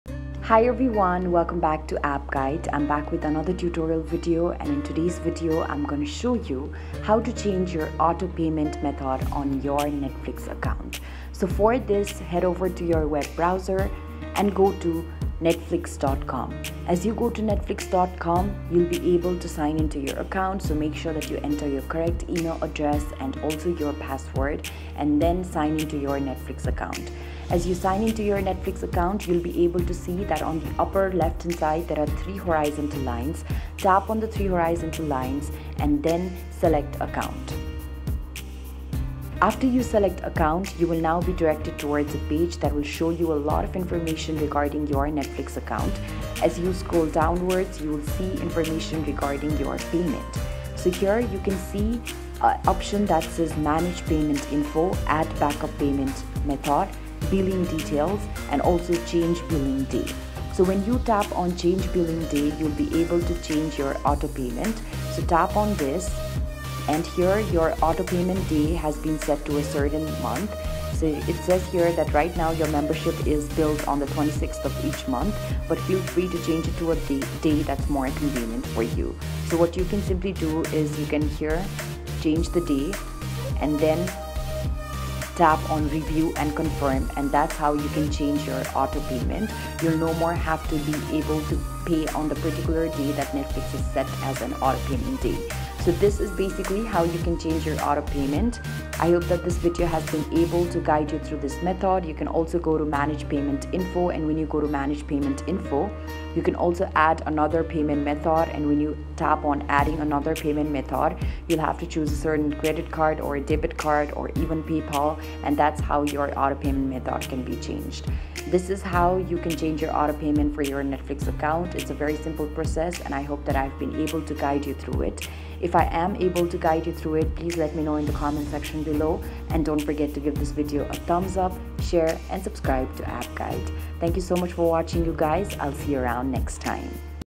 How to Change Automatic Payment on Netflix | Turn Off Auto Pay 2021
➜ Have you been wondering on how you can change auto payment on Netflix?
This video guides you in easy step by step process on how you can change auto payment on Netflix.

Follow these simple steps:
1.Open a browser and go to "netflix.com"
2.Tap on the three lined icon on the top left.
3.Tap on account.
4.Tap on change billing day.
5.Change the day.
6.Tap on review and confirm.
7.And you're all done.